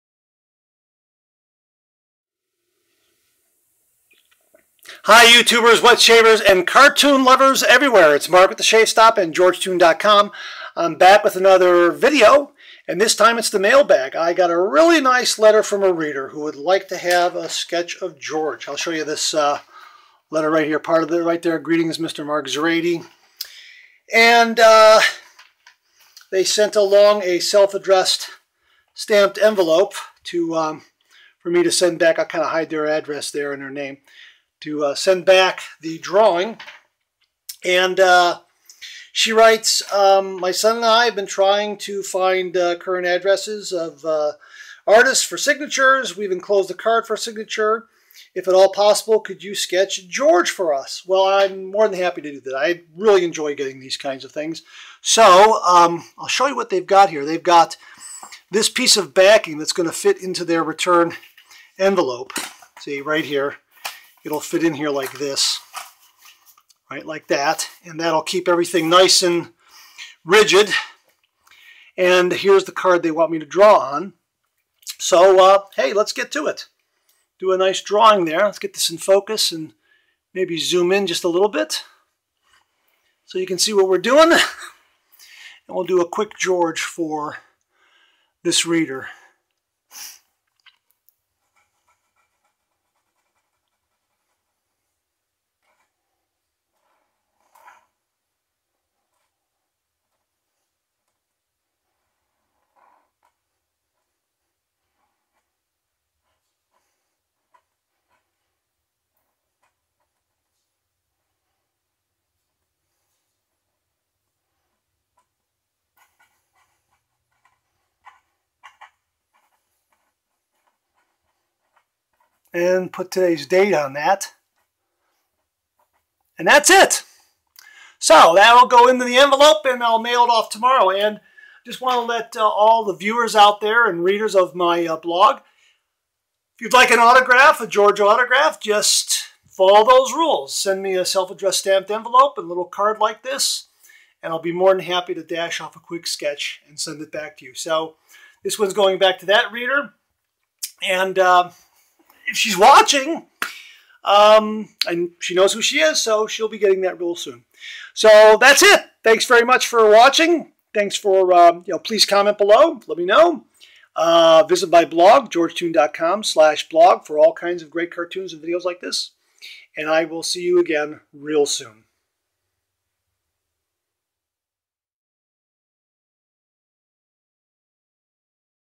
Mail Bag
I received a really nice letter from a reader.  They asked for a George autograph and I was more than happy to provide one.

The pen I used in this video can be found

Refills for the pen

I absolutely love the Tradio pen and draw all my cartoon features with it.:)

The drawing surface used in this video is a field sketch board.  I use it mostly when sitting in front of the television and doodling for funny ideas.  I cannot remember the specific brand ( I bought it years ago), but it's similar to this one